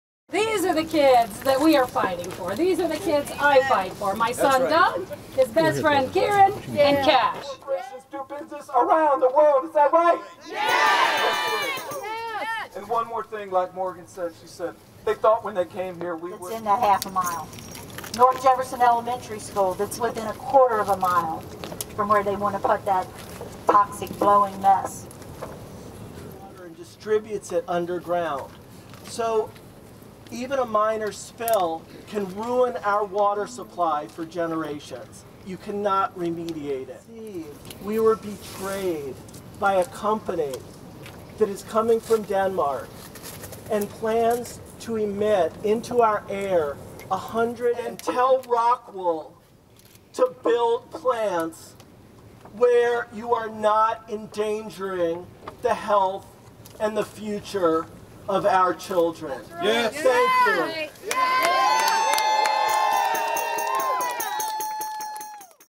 Protest against the Rockwool manufacturing plant
Environmental activists gathered at the Danish Embassy in Washington, DC to protest the construction of a potentially toxic manufacturing plant in West Virginia. Activists say the factory, which will be owned by Danish company Rockwool and will produce stone wool insulation, will harm the environment and pollute the air. If you’d like to license or purchase these clips, please log on to Pond5.com.